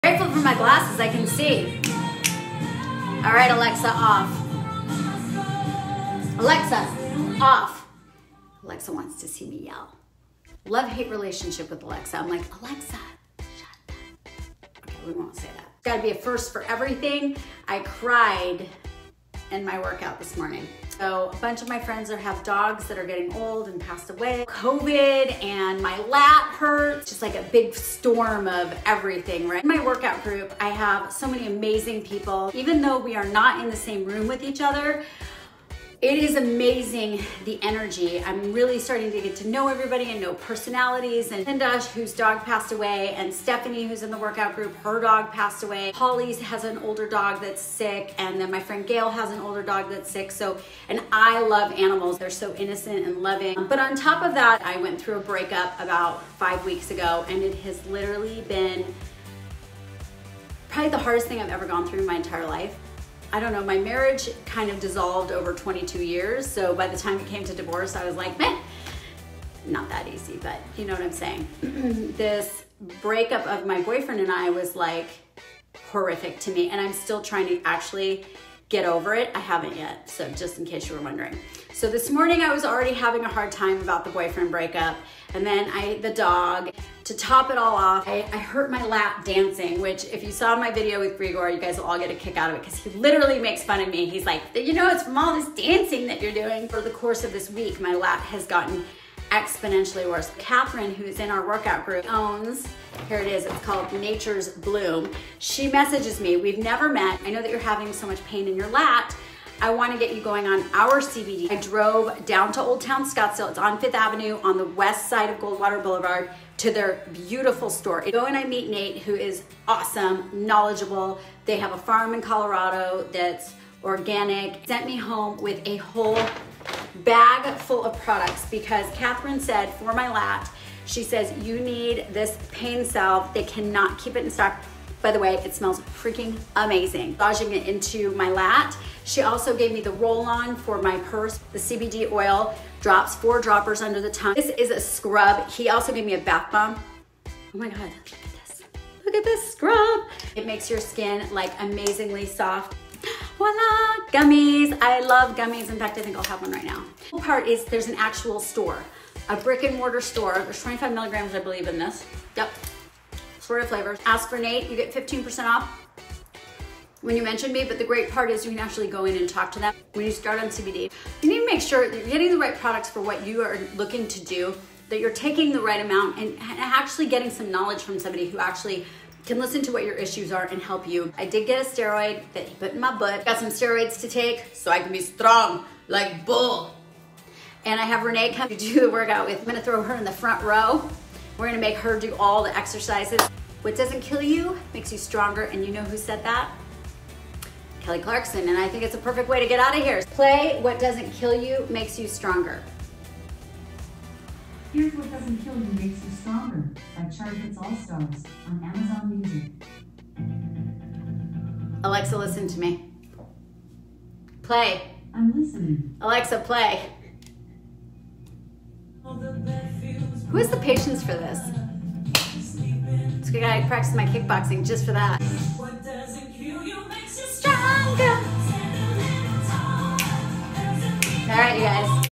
CBD... and more
So grateful for my workout group!!! Yesterday I had a very emotional workout. from my friend’s dog dying to covid to my lat injury to the breakup with my boyfriend.. I guess I hit a wall...thank u to @grigoroganyan for giving me a killer massage yesterday... save 20% when u mention lifestylejules... text for a free consultation...@drgruber_orthopedics for injecting a steroid and giving me oral steroids to reduce inflammation and to Kathryn in my workout group for loading me up with CBD from her shop @natures_bloom go enter Jules and save 15%